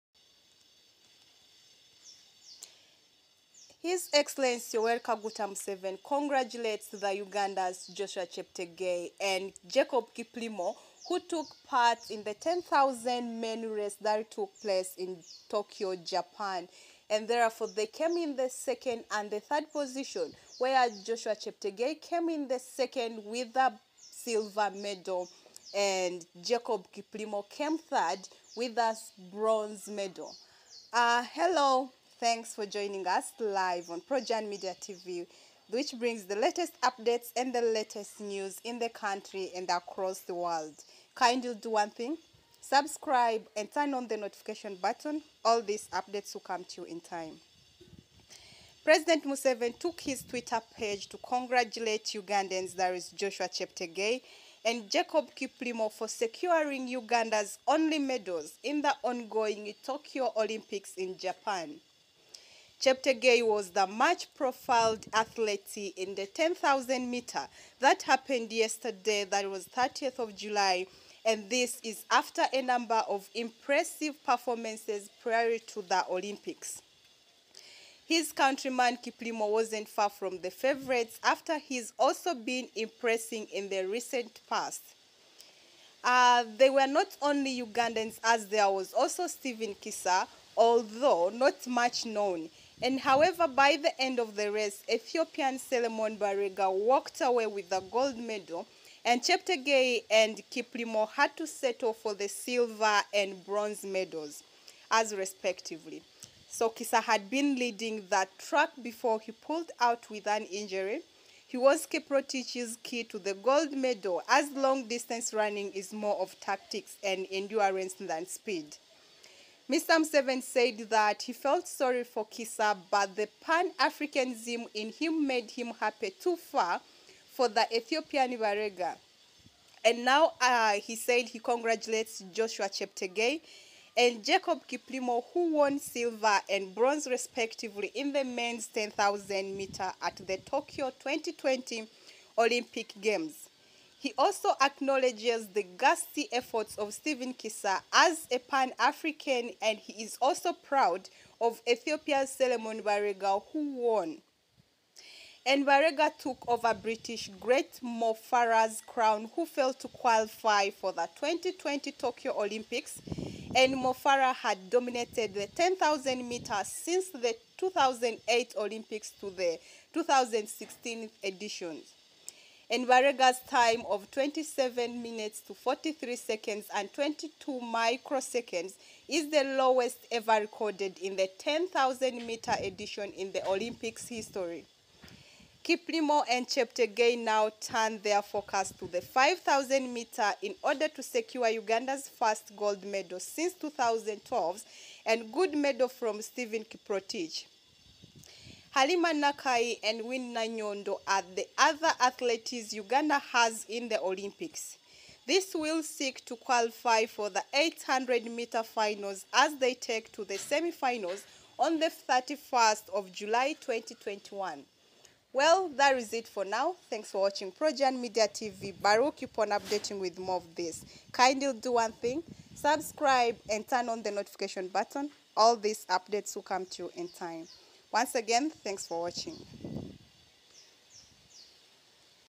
PRESIDENT MUSEVENI CONGRADULATES UGANDA'S CHEPTEGEI AND KIPLIMO.
President Museveni took to his Twitter page to congratulate Ugandans Joshua Cheptegei and Jacob Kiplimo for securing Uganda’s only medals in the ongoing Tokyo Olympics in Japan.

Cheptegei was the much-profiled athlete in the 10,000m that happened yesterday 30th July. This is after a number of impressive performances prior to the Olympics .
His countryman Kiplimo wasn’t far from the favourites after he’s also been impressing in the recent past.

They were not the only Ugandans as there was also Stephen Kissa although not much known.
However, by the end of the race, Ethiopian Selemon Barega walked away with the gold medal. Cheptegei and Kiplimo had to settle for the silver and bronze medals respectively. Kissa had been leading the track before he pulled out with an injury.

He was Kiprotich’s key to the gold medal as long-distance running is more of tactics and endurance than speed. Mr Museveni said that he felt sorry for Kissa but the pan Africanism in him made him happy too for the Ethiopian, Barega.

“And now, I congratulate Joshua Cheptegei & Jacob Kiplimo who won silver and bronze respectively in the men’s 10,000m at the Tokyo 2020 Olympic Games. I also acknowledge the gutsy efforts of Stephen Kissa. As a pan-African, I am also proud of Ethiopia’s Selemon Barega who won.”Mr Museveni tweeted.

Barega took over British great Mo Farah’s crown who failed to qualify for the 2020 Tokyo Olympics. Mo Farah had dominated the 10,000m since the 2008 Olympics to the 2016 edition.

Barega’s time of 27:43:22 is the slowest ever recorded in the 10,000m edition in the Olympics history.

Kiplimo and Cheptegei now turn their focus to the 5000m in order to secure Uganda’s first gold medal since 2012’s good medal from Stephen Kiprotich.

Halima Nakayi and Winnie Nanyondo are the other athletes Uganda has in the Olympics.

These will seek to qualify for the 800m finals as they take to the semifinals on 31st July 2021.